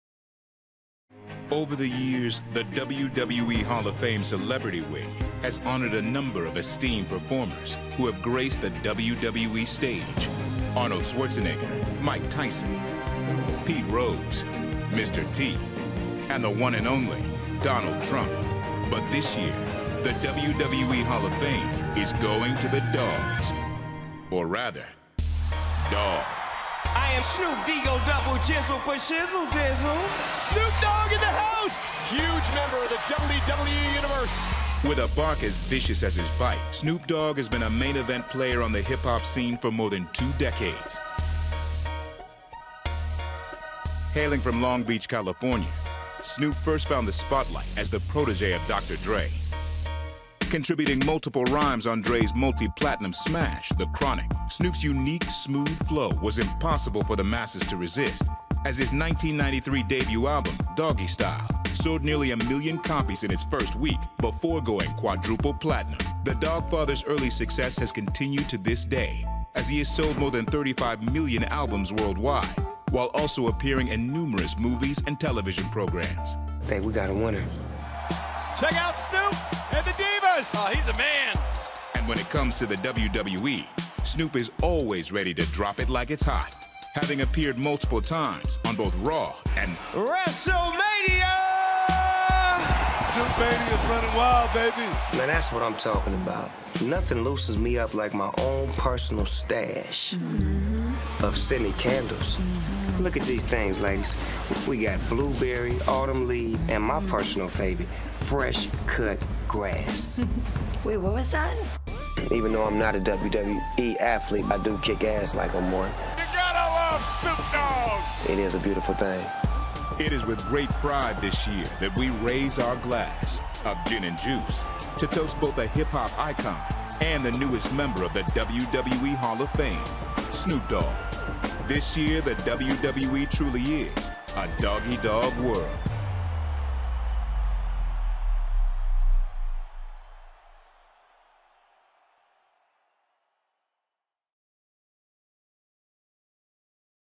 Snoop Dogg wird 2016 in die WWE Hall of Fame aufgenommen
Die Hip Hop Legende Snoop Dogg wird am Vorabend von WrestleMania 32 in die WWE Hall of Fame eingeführt.